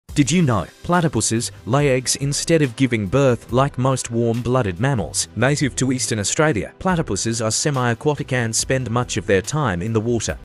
Interesting little Platypus
The platypus is indeed a unique and fascinating creature. It's one of the few mammals in the world that lays eggs, rather than giving birth to live young.

The platypus is native to eastern Australia, including Tasmania, and it's renowned for its extraordinary blend of features. It has a duck-bill, webbed feet, and lays eggs like a reptile, yet it's a warm-blooded mammal. Platypuses are semi-aquatic and spend much of their time in water, where they hunt for small aquatic creatures like insects, worms, and crustaceans.

Female platypuses typically lay one to three eggs at a time, which they incubate by curling their bodies around them. After about ten days, the eggs hatch, and the mother feeds her newborns with milk produced by mammary glands through openings in her skin. The milk pools in grooves on her abdomen, and the platypus babies lap it up directly from her skin.

The platypus's egg-laying and milk-feeding characteristics make it a remarkable example of nature's diversity and adaptation. It's often considered an iconic symbol of Australia's unique wildlife and is a subject of fascination for scientists and nature enthusiasts worldwide.